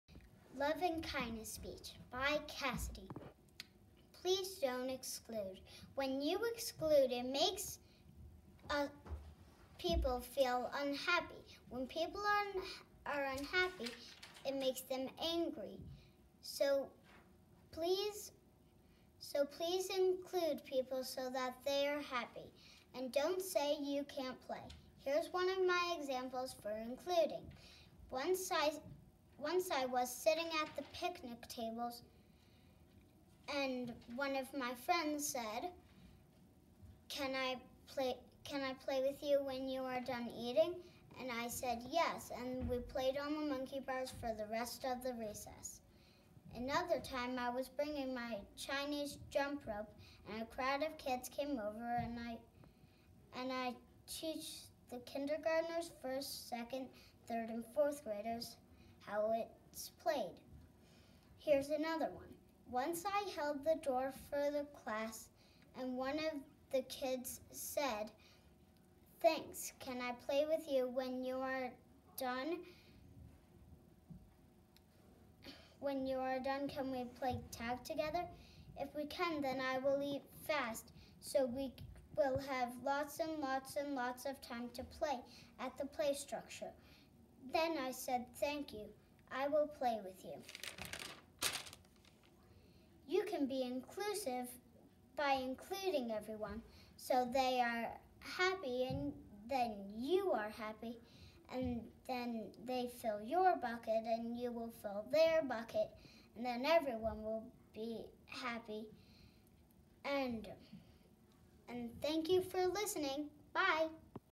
Kindness Can Change A Nation – Kindness Speech By 8 old.
Hi, I’m Cassidy and this is my submission for the Kindness Can
Change A Nation – National Kindness Speech Contest, presented by Think
Kindness & Top Youth Speakers.
I think kindness is important because it can change the world.